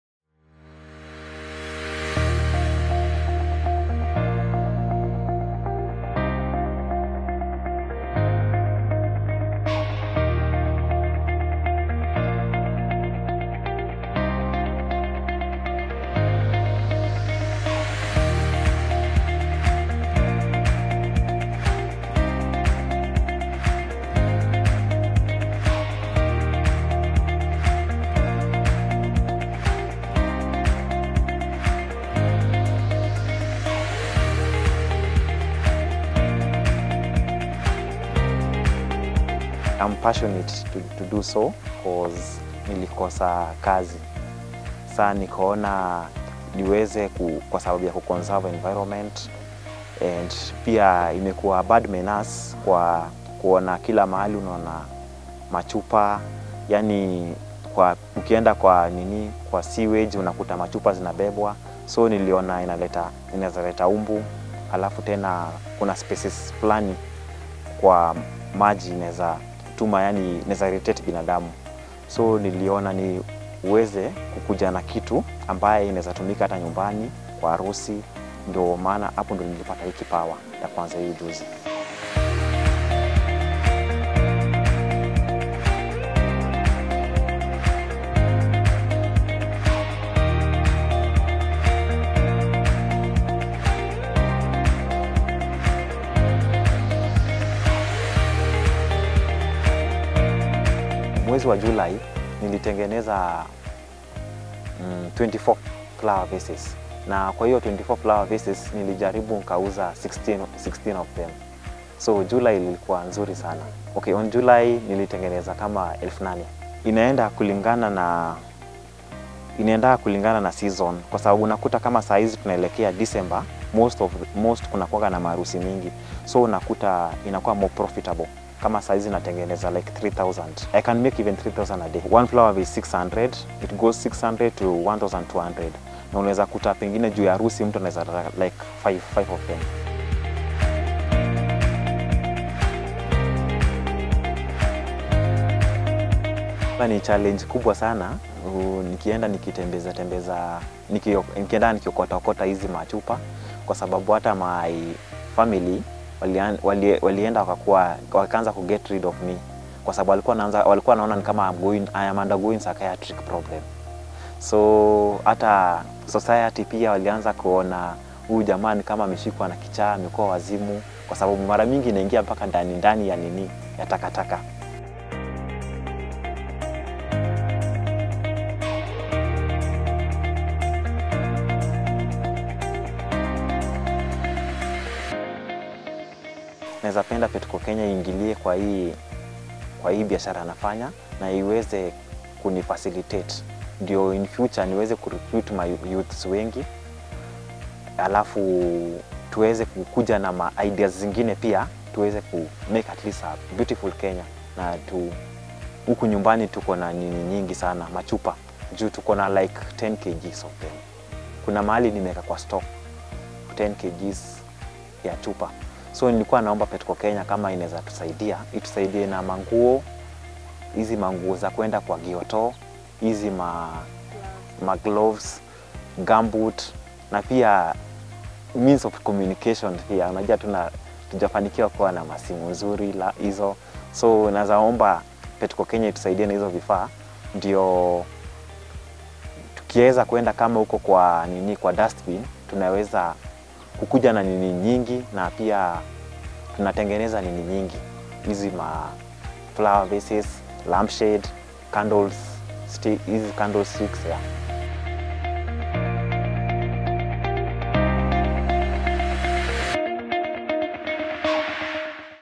Trash to treasure
In an effort to recycle plastic bottles and reduce the environmental effect of the material,one Kenyan Davis Oduor and his friends has opted to collect bottles, craft them into impressive shapes and paints them.
The end results of these plastic bottles are flower vases, lampshades and candle stands which are later sold and used for interior decoration.